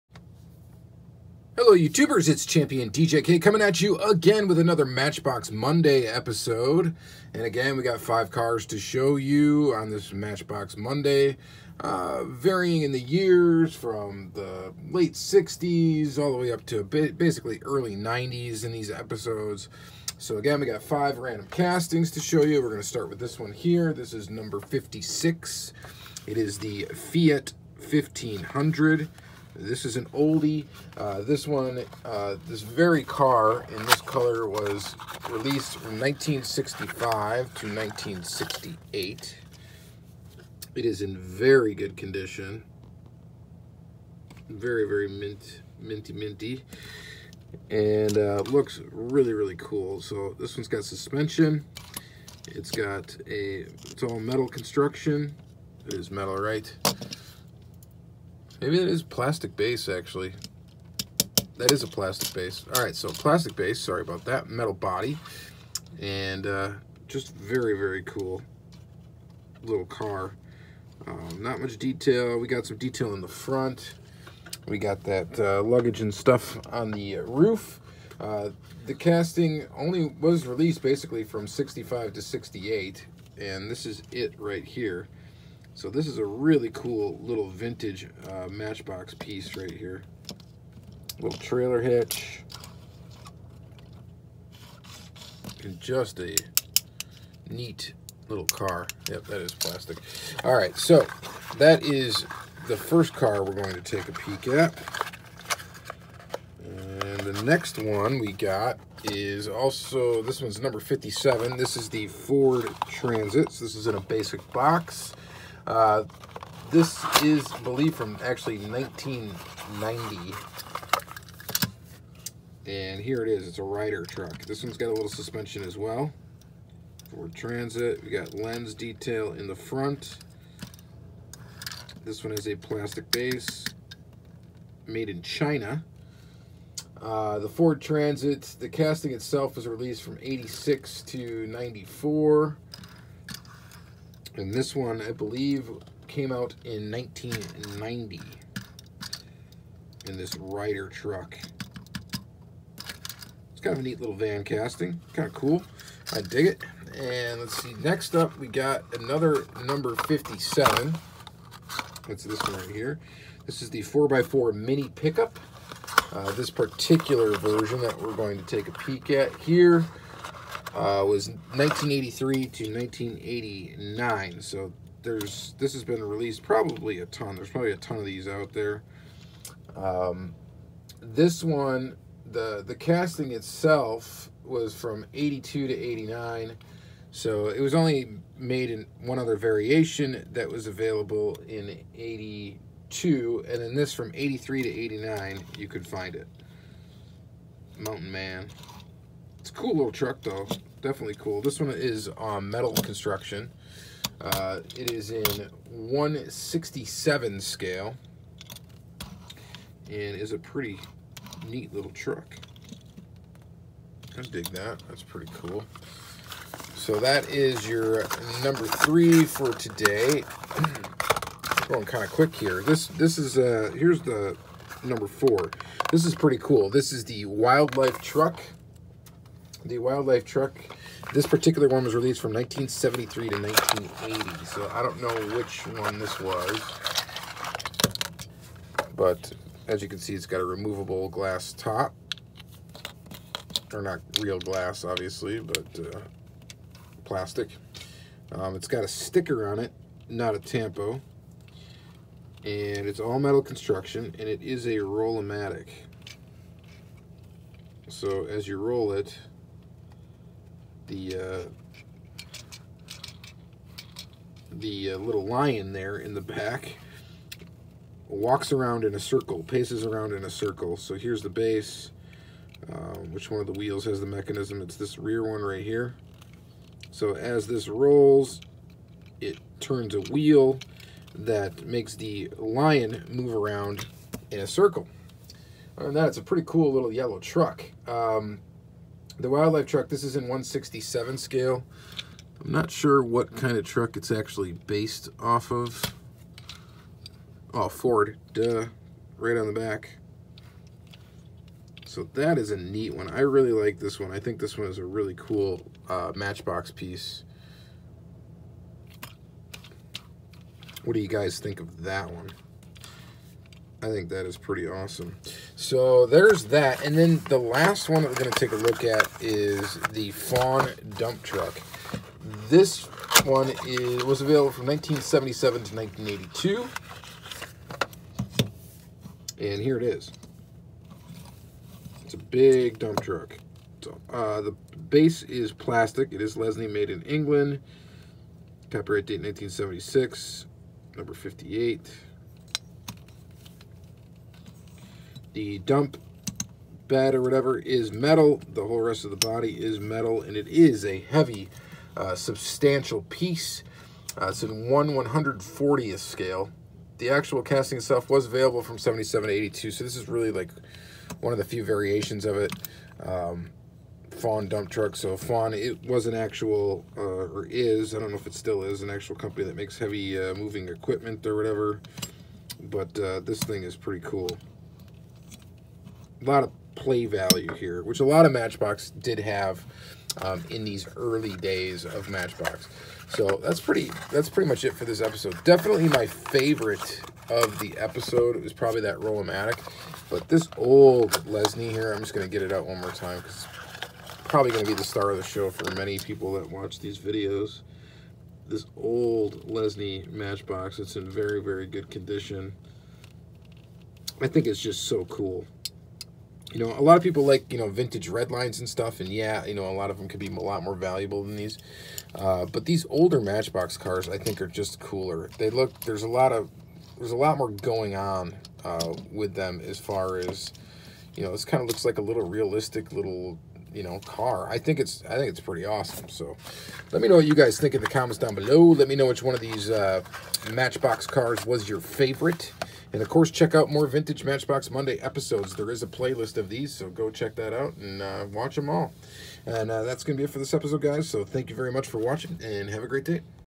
Vintage Matchbox Monday Ep 18
56 Fiat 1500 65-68 Green 30
57 Ford Transit  90 Yellow Ryder 5
57 4x4 Mini Pick-up 83-89 Blue 5
57 Wild Life Truck 73-80 Yellow 17
58 Faun Dump Truck 77-82 Yellow  5

I try to keep all of my videos organized in playlists to make my channel easy to navigate here are some to check out.